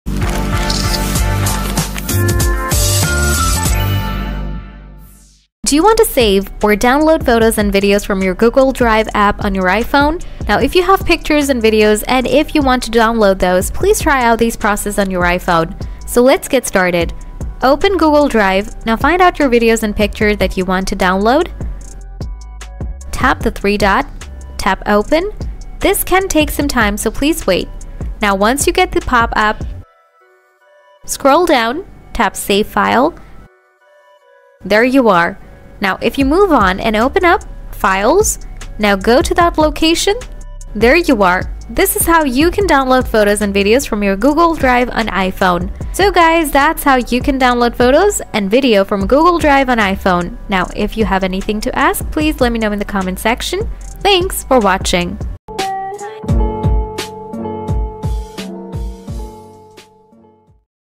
How to Download Google Drive Photos & Videos to Your iPhone
Do you want to download any photos and videos from Google Drive to the iphone gallery? Watch this video to Save photos and videos from google drive to your iphone.
Many iPhone users are asking how they can download photos from google drive or videos from google drive. Now if you want to Download google Drive photos and videos on your iphone and also want to Save photos and videos from google drive to your iphone, then stay tuned!
So, after getting questions here, I will show you how to download photos and videos from google drive to your iphone.

00:01- Video intro
00:15- Step 1: Open the drive and go to the file
00:24- Step 2: Follow these steps to download
00:55- Solution ending